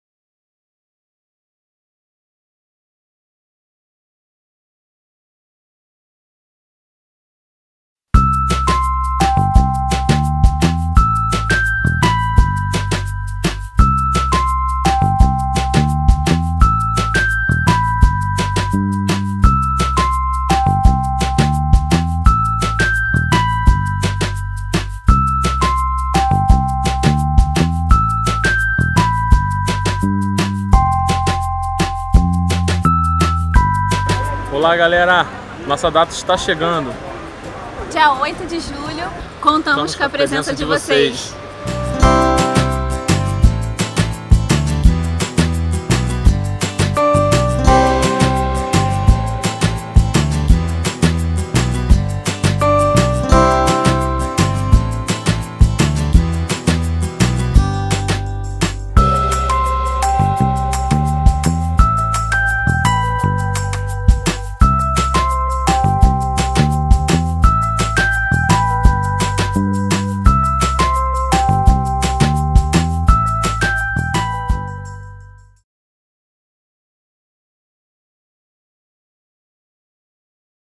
Jeniffer e Hugo - Save the Date
Convite digital para o Casamento de Jeniffer e Hugo que será realizado dia 08/07/2017
Créditos para Casal Figueira PRODUÇÕES